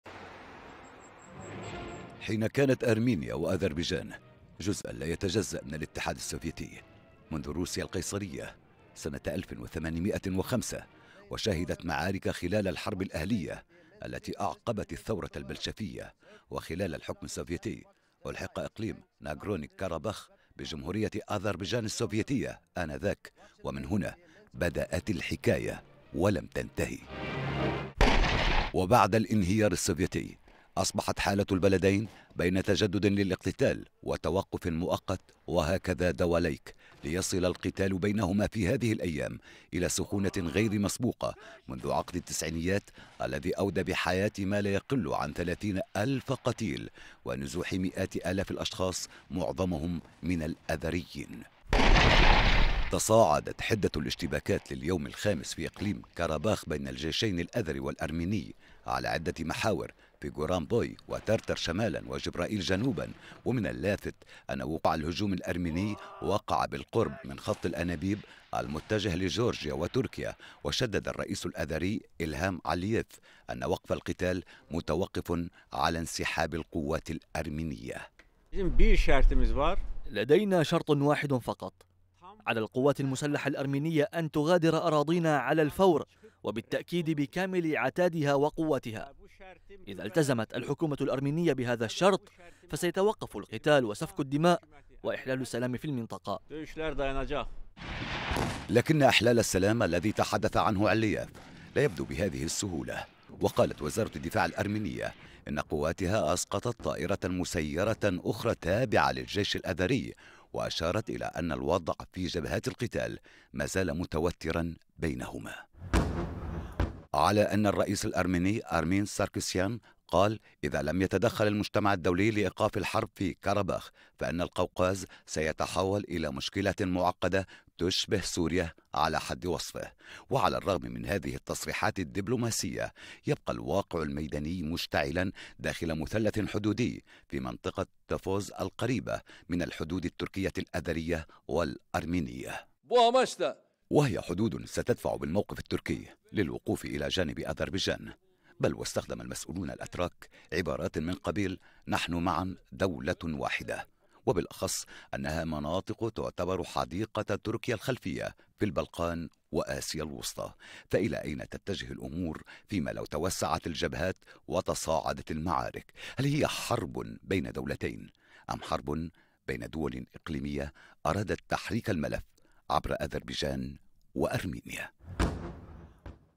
الصراع الأذري الأرميني يتجه إلى التصعيد
هوتبيرد HD
V 12520